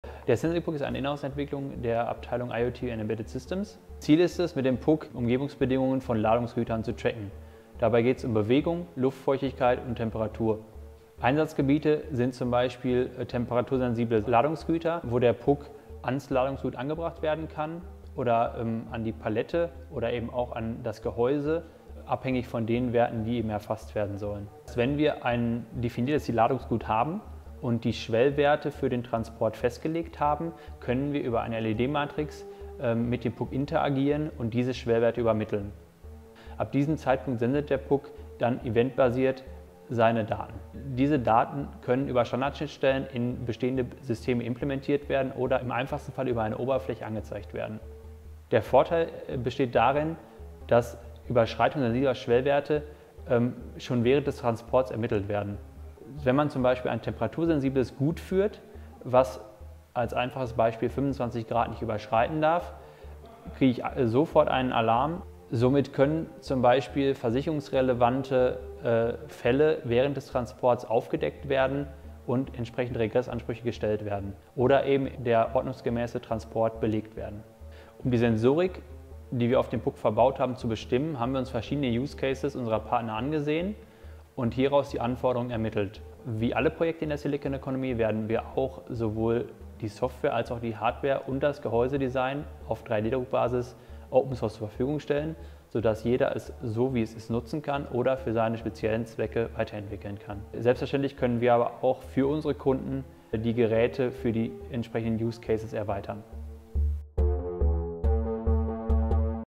Silicon Economy Entwicklungsprojekt »Modulare Open Source IoT-Devices« | »Sensing Puck«
Klein, kompakt, robust: Das Tracking-Tool »Sensing Puck« – jüngste Erfindung aus dem Entwicklungsprojekt »Modulare Open Source IoT-Devices« – zeichnet sich dadurch aus, dass er unkompliziert und einfach in bestehende Prozessketten integriert werden kann.

Mit dem »Sensing Puck« können Versandeinheiten entlang der Transportkette vom Warenausgang bis zur Warenübergabe ohne jegliche zeitliche und informatorische Lücke getrackt werden. In dem Gehäuse des Geräts können verschiedenste Sensoren zum Einsatz kommen. Je nach Sensibilität des Transportguts ist die Messung der Temperatur und Luftfeuchtigkeit mit geringen Reaktionszeiten möglich. Darüber hinaus ist eine Lagedetektion möglich, sodass sich z. B. Kippvorgänge erkennen sowie Beschleunigungsprofile aufzeichnen und analysieren lassen. Die Kommunikation erfolgt energieeffizient über bekannte WLAN- und Mobilfunknetze – sowohl innerbetrieblich als auch außerbetrieblich.

Die Baupläne für das kostengünstige Device stehen, wie alle Entwicklungen in der Silicon Economy, Open Source zur Verfügung.

Dieses Projekt wird durch das Bundesministerium für Digitales und Verkehr gefördert.